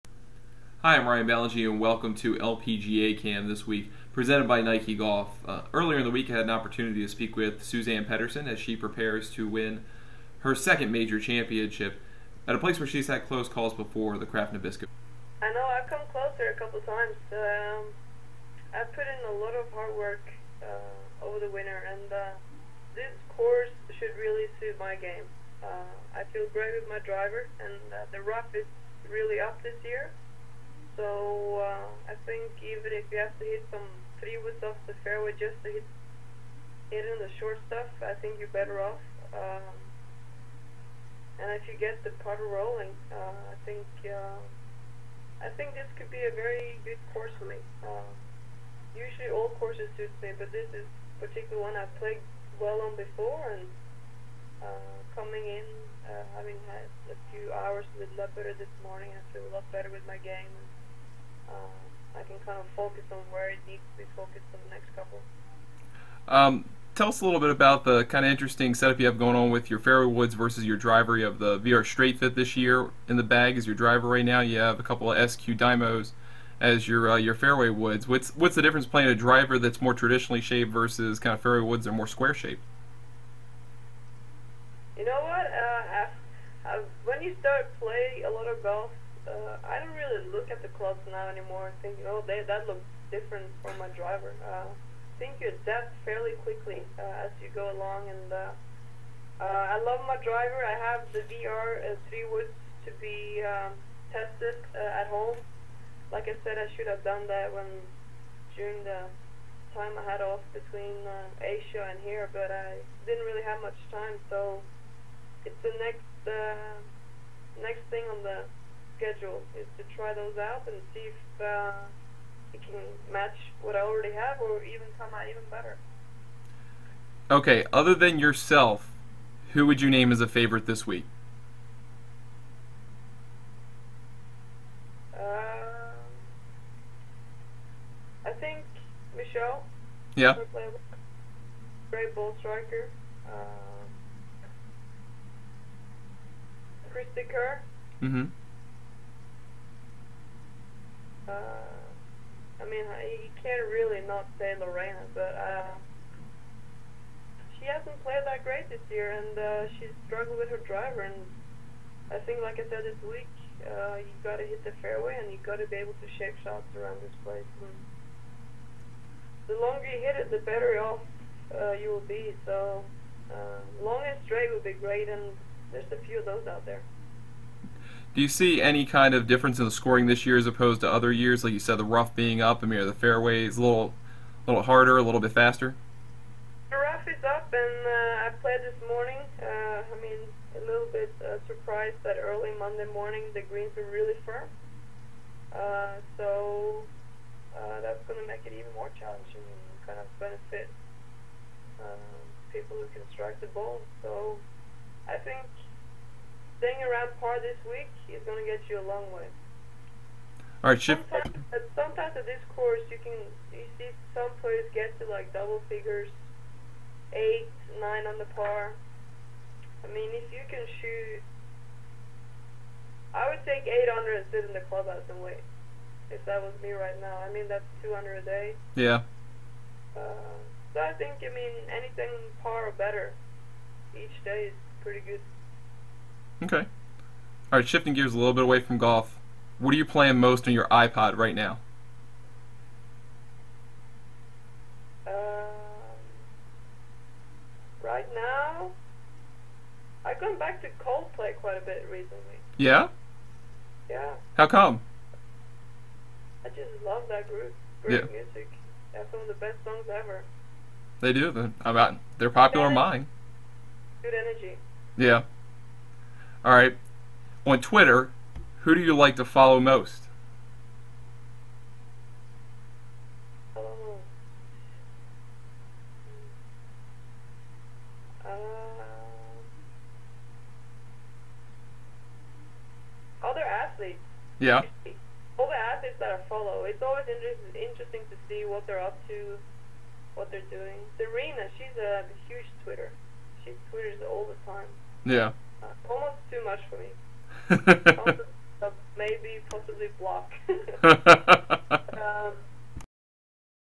LPGA Cam with Suzann Pettersen from Kraft Nabisco Championship
Waggle Room's Ryan Ballengee speaks with Nike Golf staff member Suzann Pettersen as she prepares for the LPGA Tour's first major of the year at the Kraft Nabisco Championship.